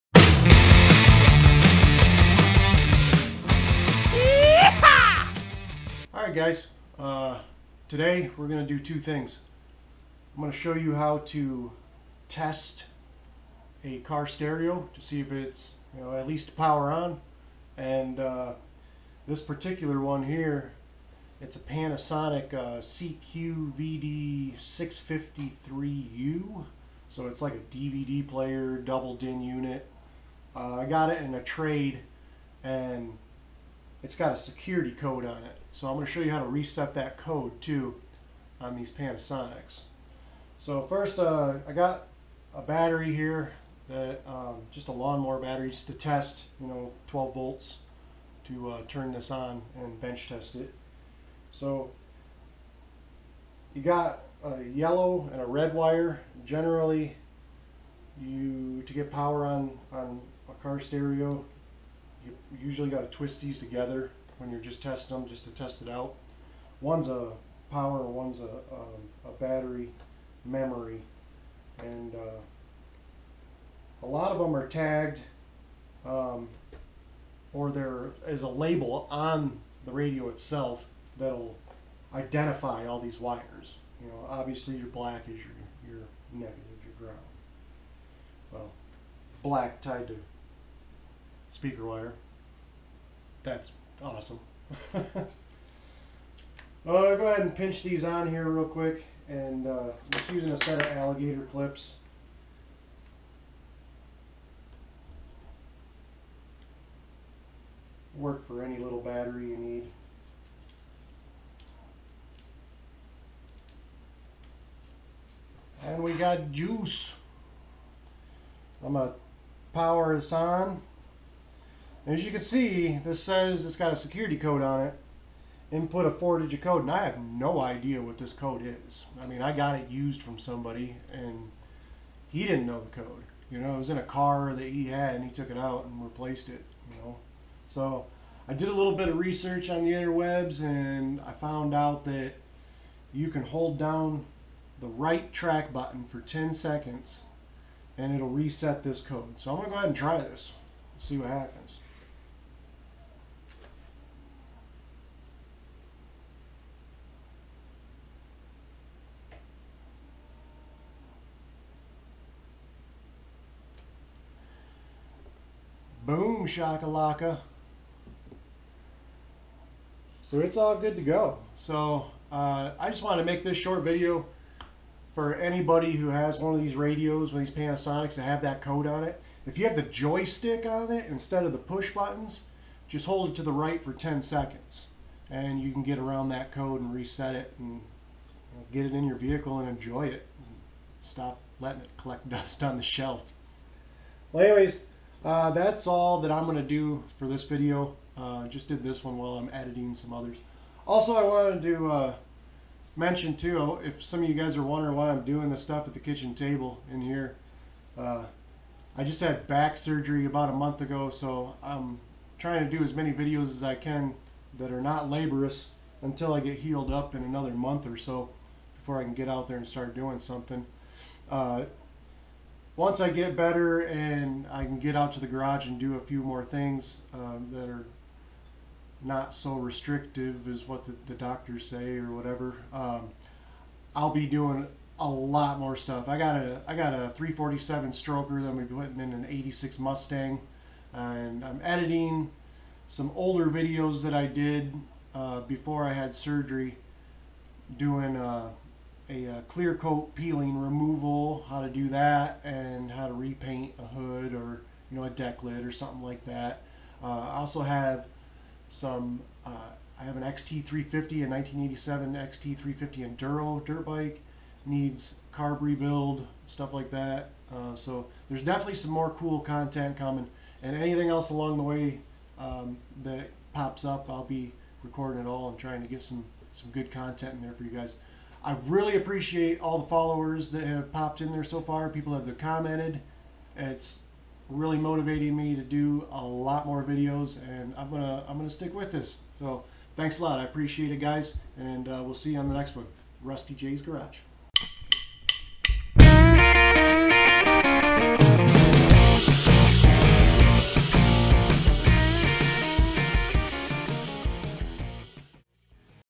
Panasonic security code hack
how to bench test a car radio, and how to get around the security code on Panasonic car stereo head units